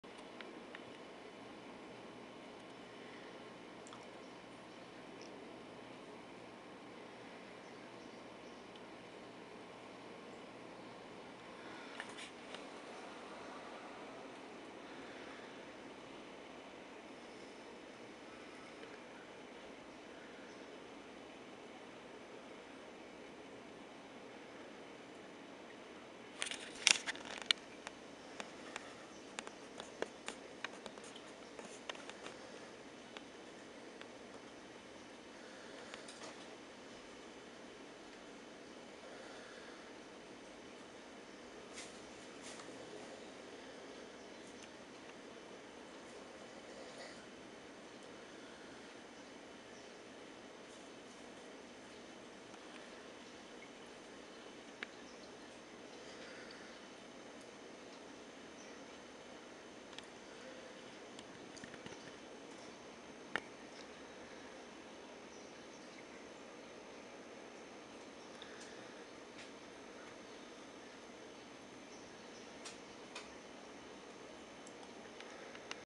Engineer Goby Hard At Work
diggin' all day err day

lol, this little guy builds new tunnels in the sandbed every morning.